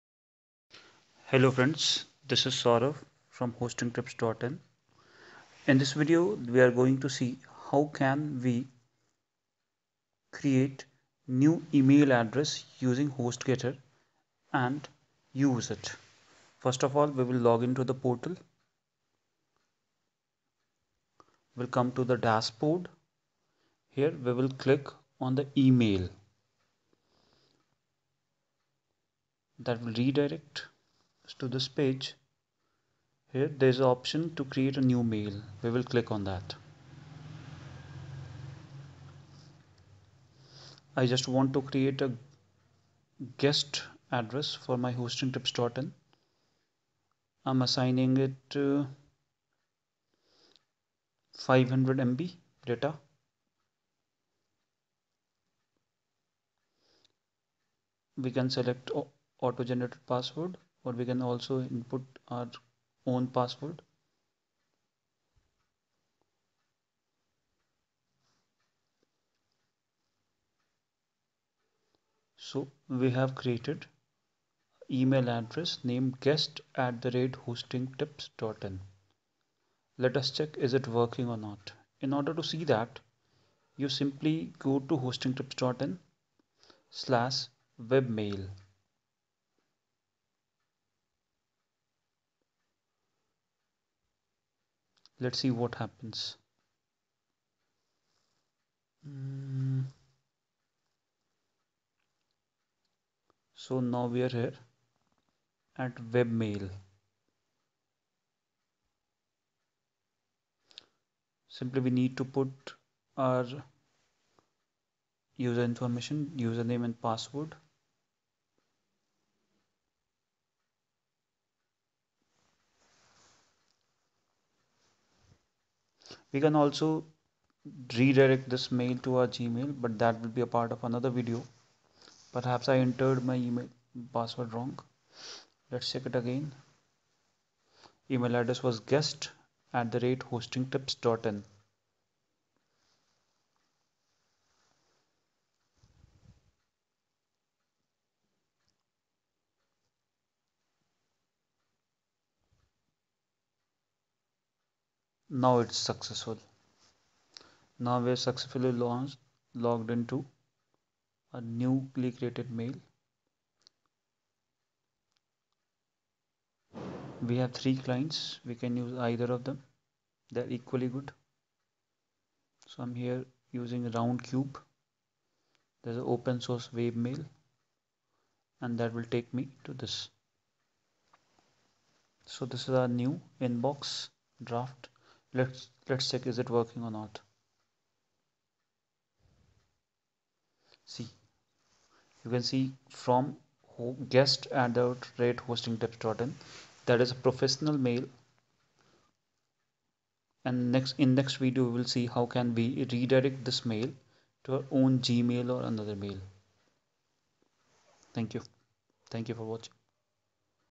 How to Set Up Hostgator Email 2020 for Domain
Hostgator  email configuration explained step by step explained in 4 steps. Steps to check your webmail.